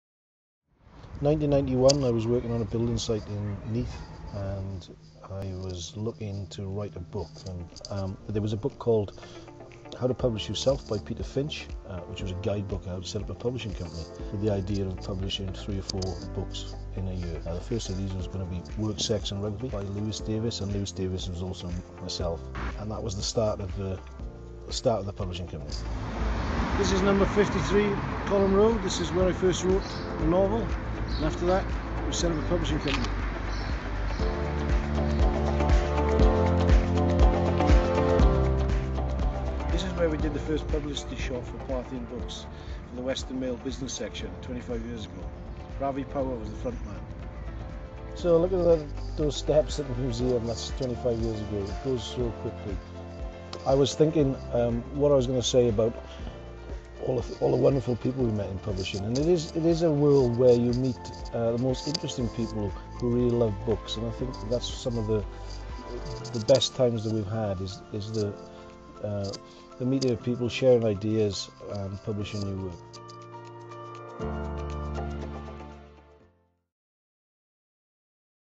Parthian Turns 25
Parthian Books is celebrating 25 years in publishing. This video shows where we've been and where we're going.